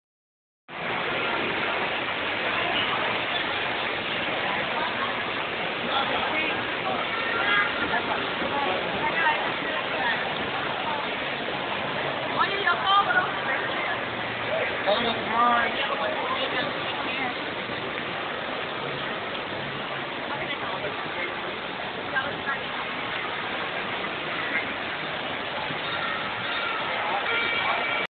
Yellow Green Farmers Market Walk Around - Fresh Florida Oranges and Grapefruit
Come visit us at the ygfarmersmarket.com More than just a Farmers Market in Hollywood, Florida